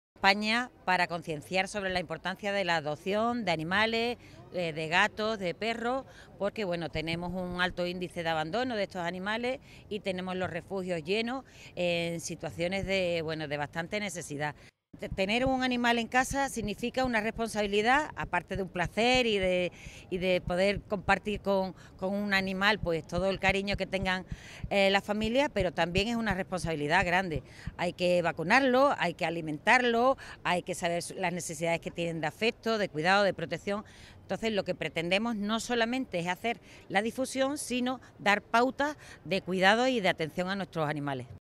El Ayuntamiento de Cádiz inicia una campaña para promover la adopción de animales
Bajo el lema, 'Date vida, dale vida' se pide responsabilidad ante el alto índice de abandono de animales y la situación en la que se encuentran los refugios